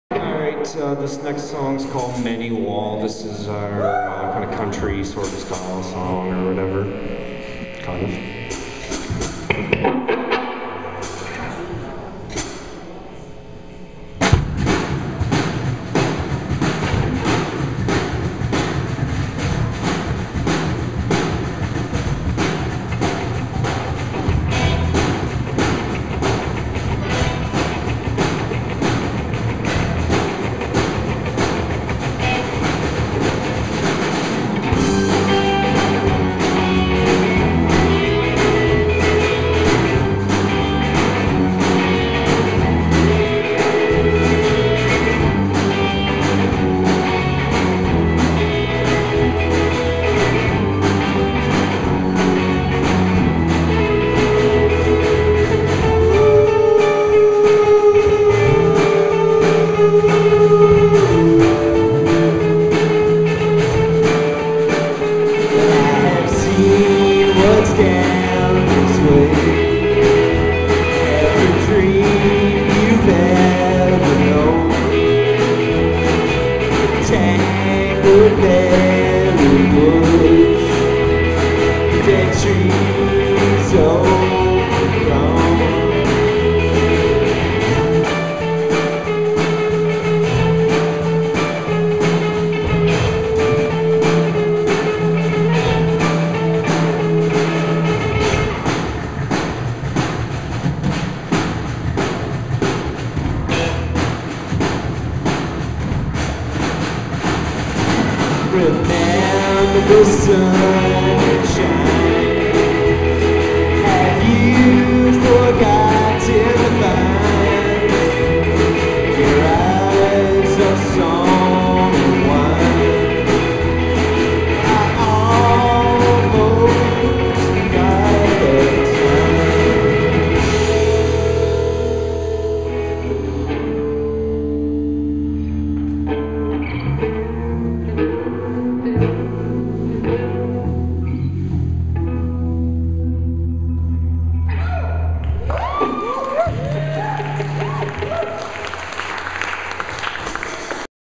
CORNER ORGANS-OFF - Mini Wall
Set To 720p for HD audio/video
Adam LeDrew, Mitchell Carree, Matt Trefry, Justin Crowe & Justin Murphy
At the Celtic Circle Cultural Centre